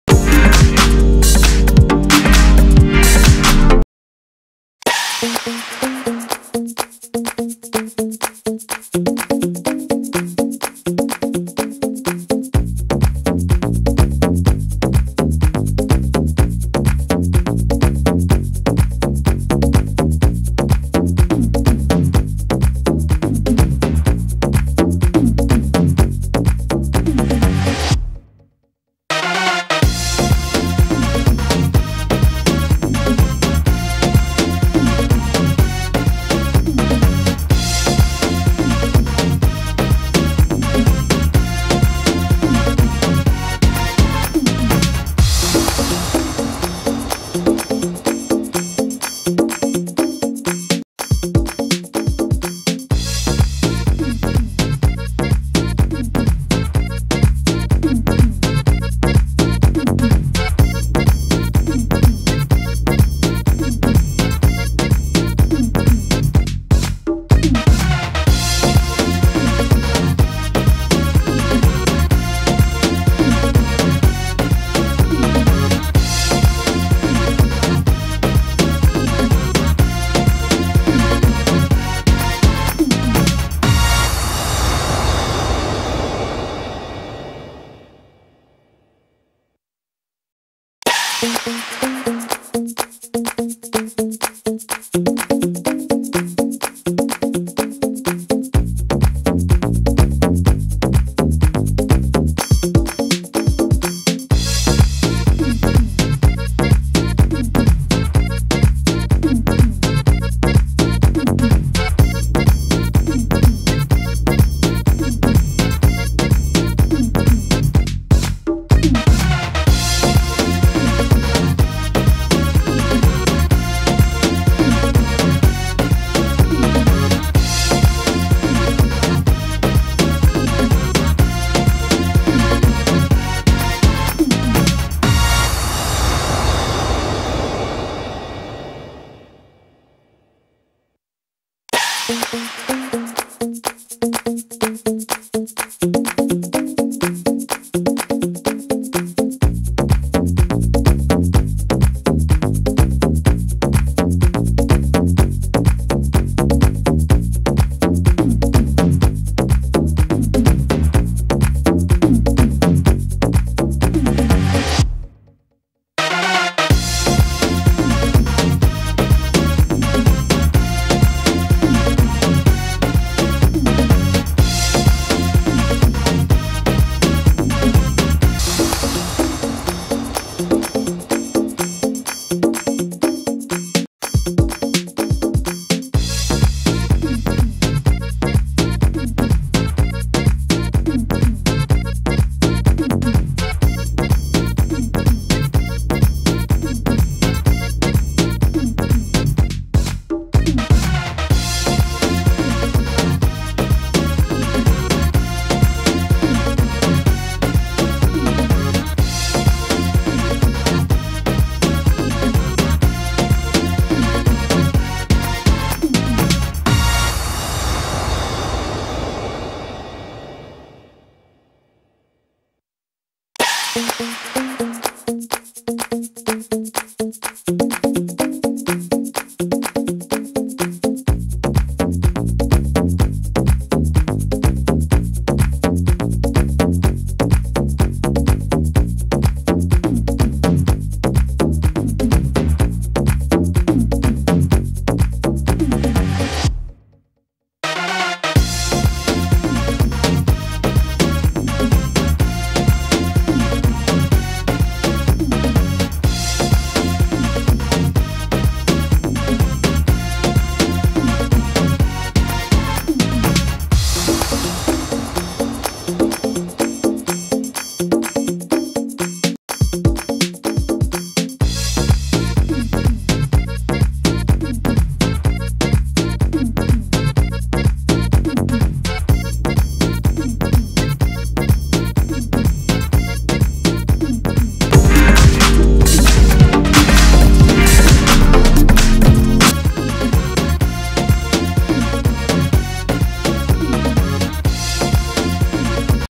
iPhone 11, iPhone 11 Pro, Apple Watch 5 all details and prices
iPhone 11, 11 Pro, Apple Watch all details from Apple event in 5 Mins

Dear Friends,

Have a quick look on Apple event where they have launched iphone11 iphone11Pro & new
All info in just 5 Mins